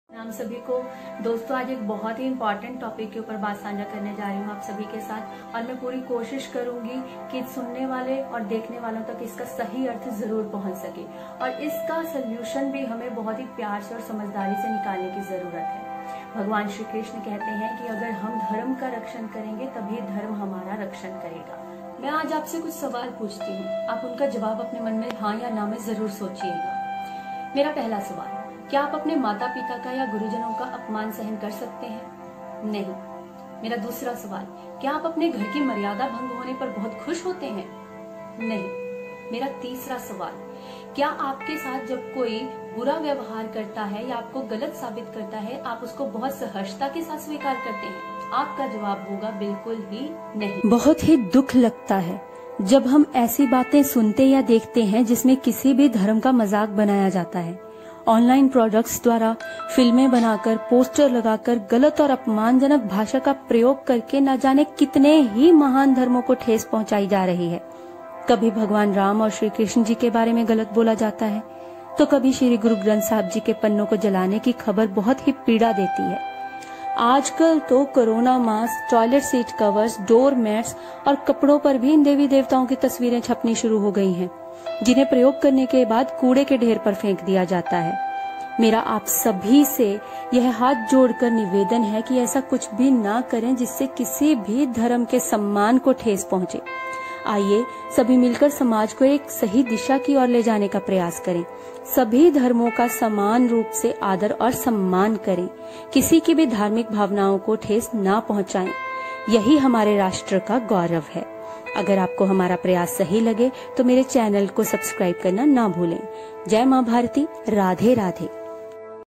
Raise voice | Stop hurting religious sentiments | Respect | PeP Talks | Poonam Sharma
Deliberate and malicious intention of outraging the religious feelings , with this video . I request Indian people to please pay attention and  raise their voice against such products , entertainment , online shopping who are selling product with picture of God , hindu gods .
Religious sentiments ,Boycott products display picture , Join hands .Religious debate God is one , Respect all religions , spread love and peace in the nation ,religious festivals